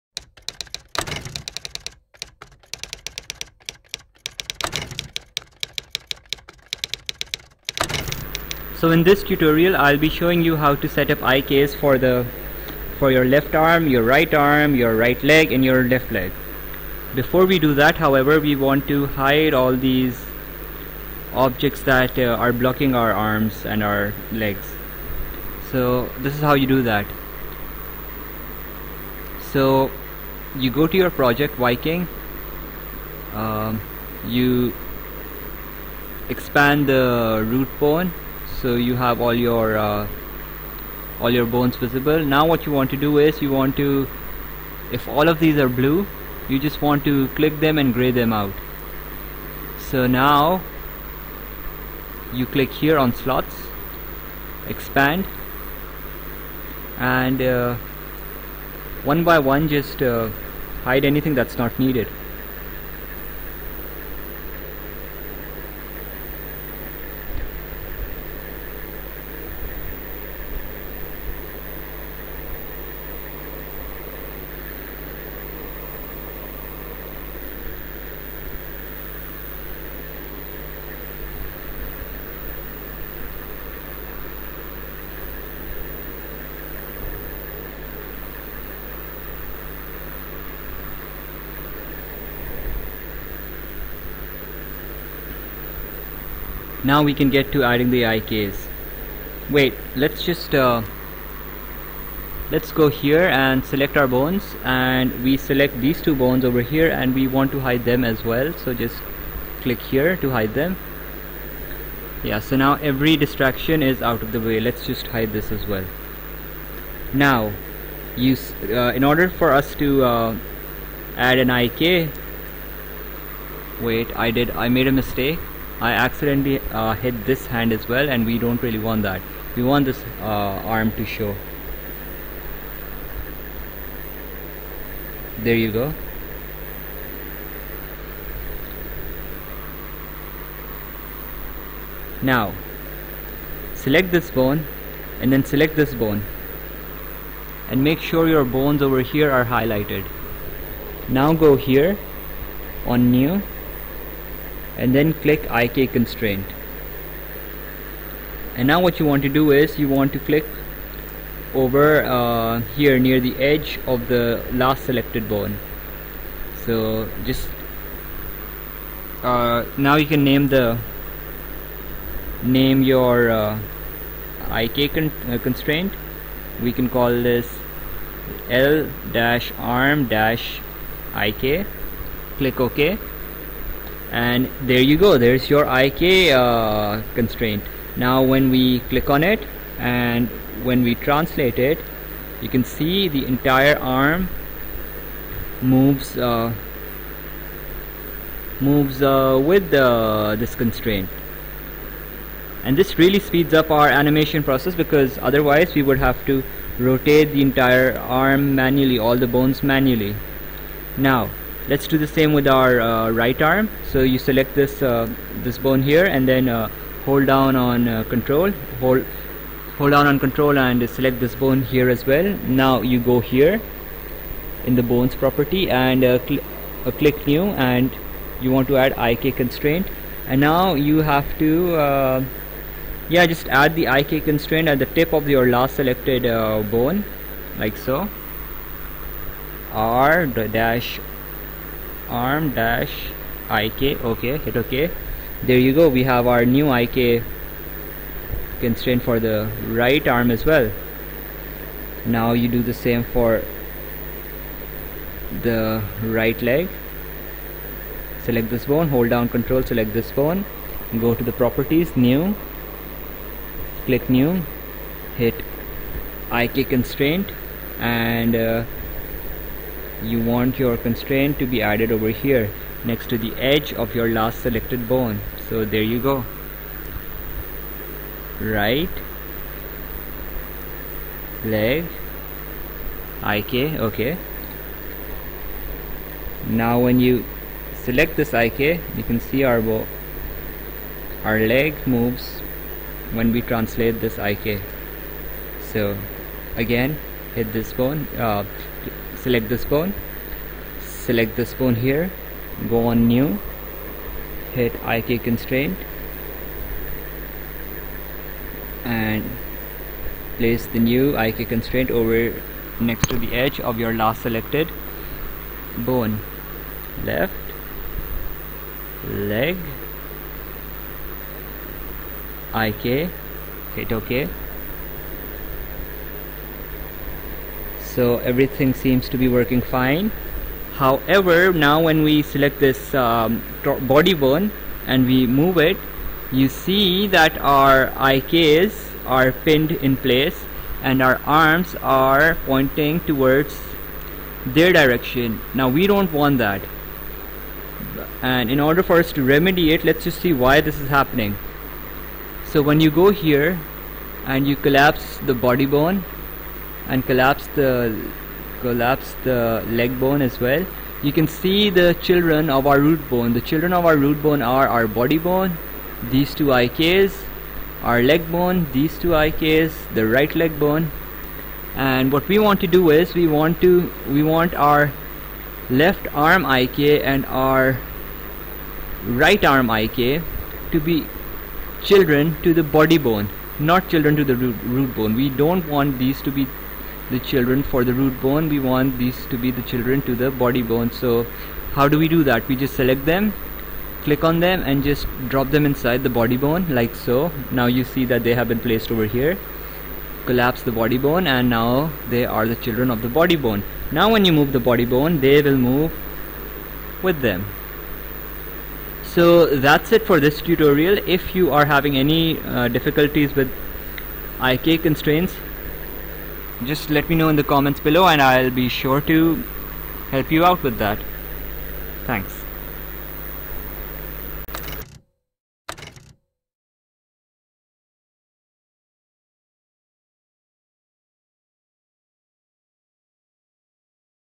Starting out with Spine; Pt 5 Applying IK Constraints
Starting Out With Spine; An Animators Guide To Understanding The Basics Of Spine Pro
In the fifth video of this series we see how we set up IK constraints and how they facilitate us while we pose or animate our project...